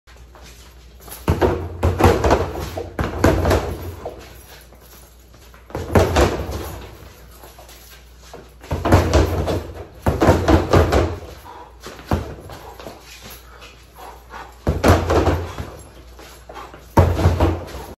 Boxing update 12-2-2022
Made some major changes that may not be easy to see. For one, I’ve started sparring so it put some reality into my movements. Also planted my back foot in my punches which was a significant improvement in stability and power. Obviously it’s nowhere near perfect or clean as yet but just documenting my progress here as usual. 🥊